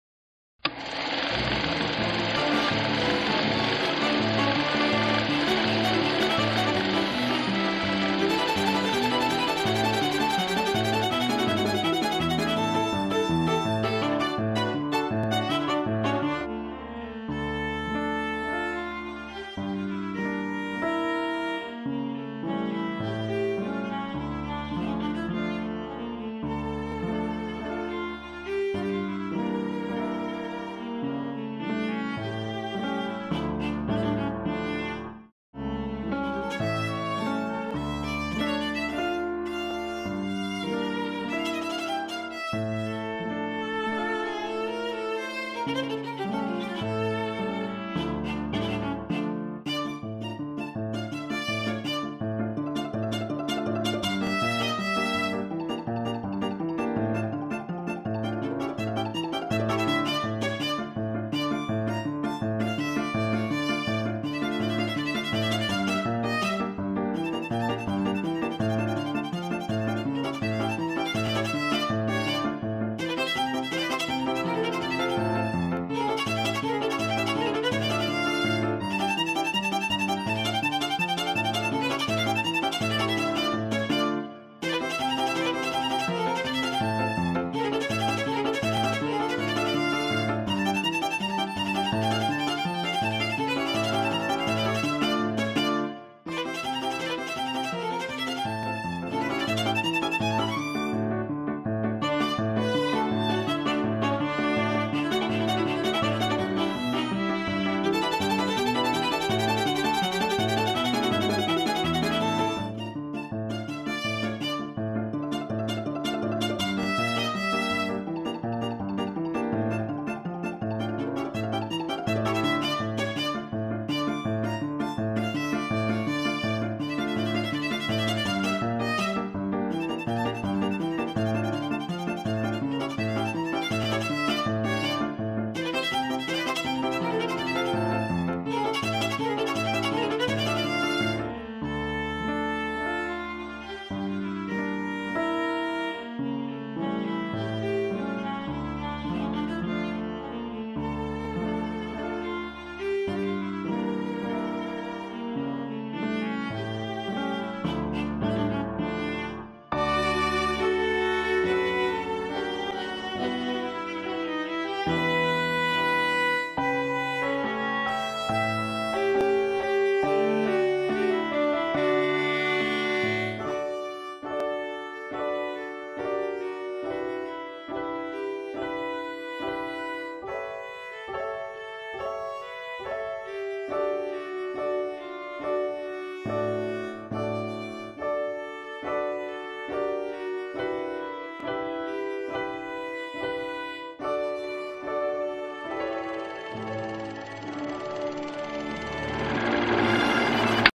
Silent Western Short
A short film created to run as part of a stage play based on old Western historical figures.